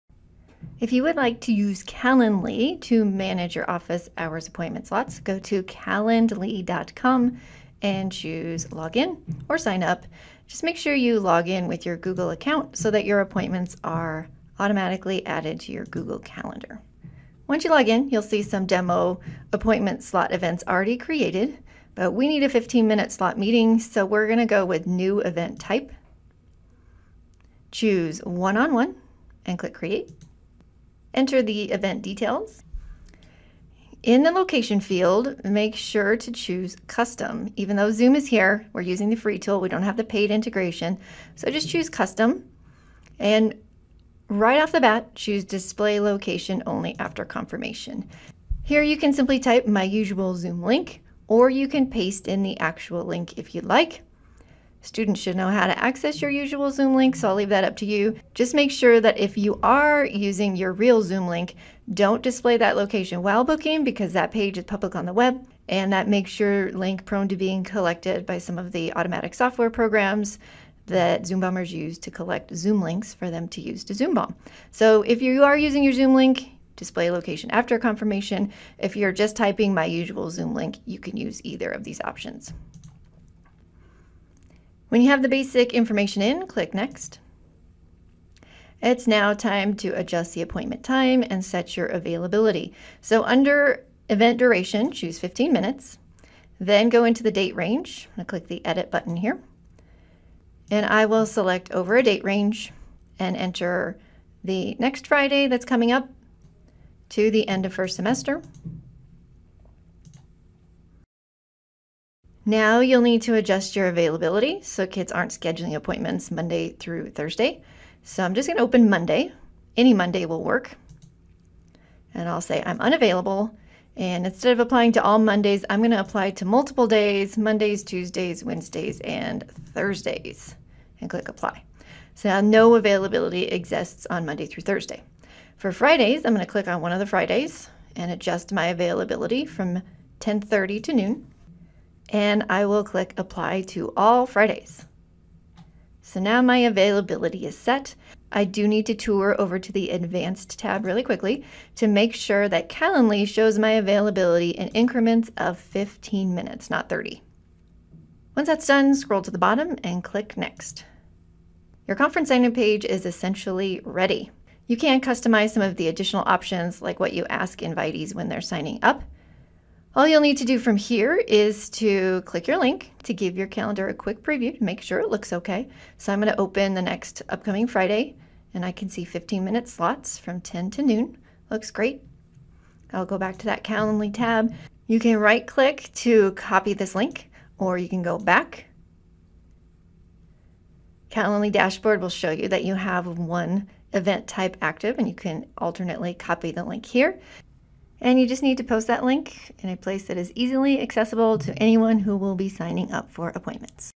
Calendly (Free) for Office Hours Appointments
Use Calendly to set office hour windows and appointment slots.

We have office hours on Friday mornings, so this video was created for my staff, but the process is generalizable to whenever you hold office hours, whether that is one specific day or multiple days/week.